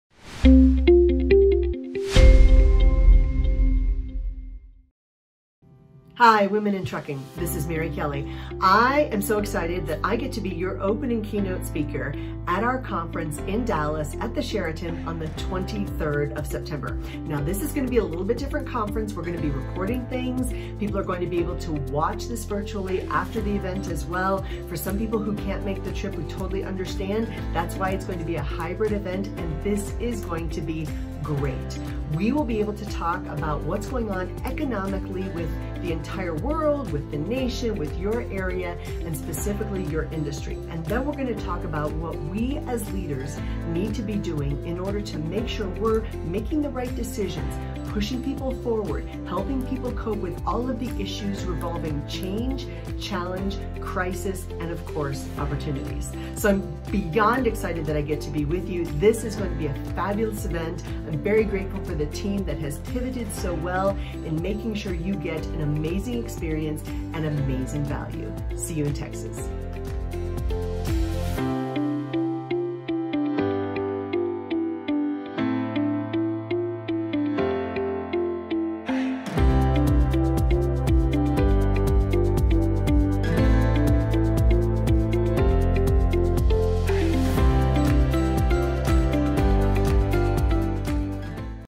Mary Kelly Opening Leadership Program for the Women in Trucking Conference
Join former Naval Intelligence Officer, Commander Mary Kelly, for a fun and entertaining approach to being a better leader.
Author of 15 books on leadership and business growth, Mary loves building stronger organizations through leadership development.
With a PhD in economics, Mary understands how to improve profit growth, even during times of challenges, changes, and crises.  Mary focuses on  finding opportunities and identifying future customer needs.